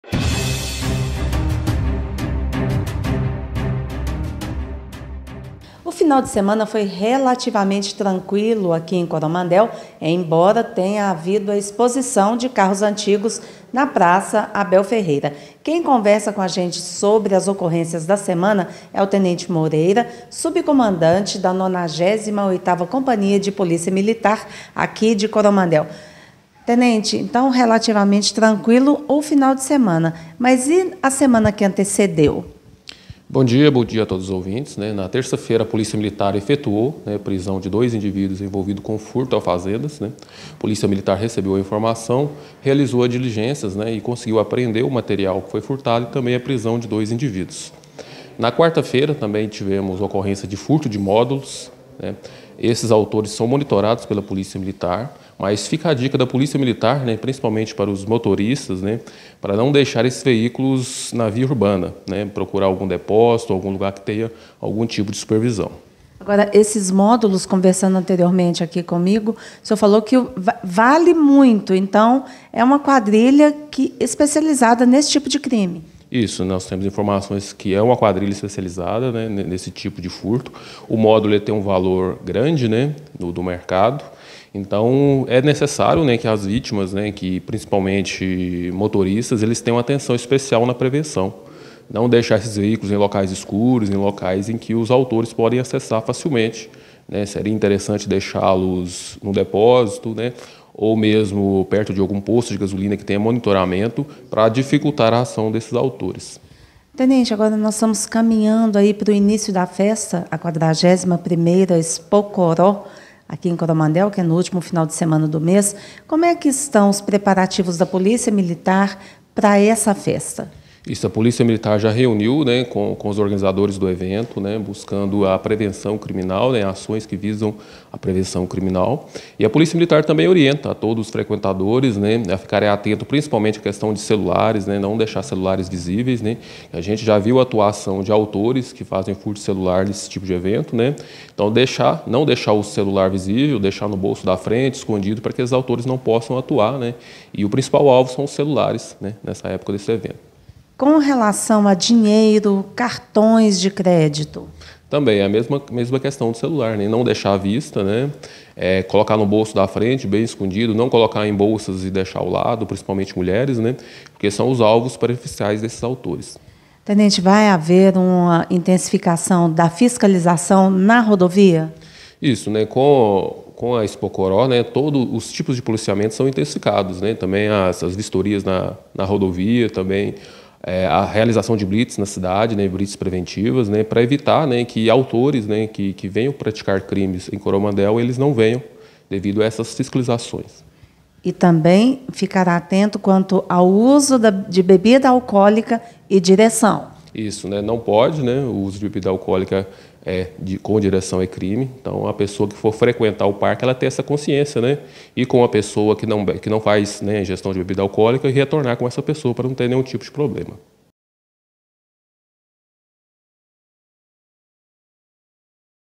PM faz resumo das ocorrências em Coromandel
Destaque para mais um furto de módulo de caminhão ocorrido na quarta (12) e para as dicas da 98ª Cia de PM de Coromandel para a Expo Coró 2023.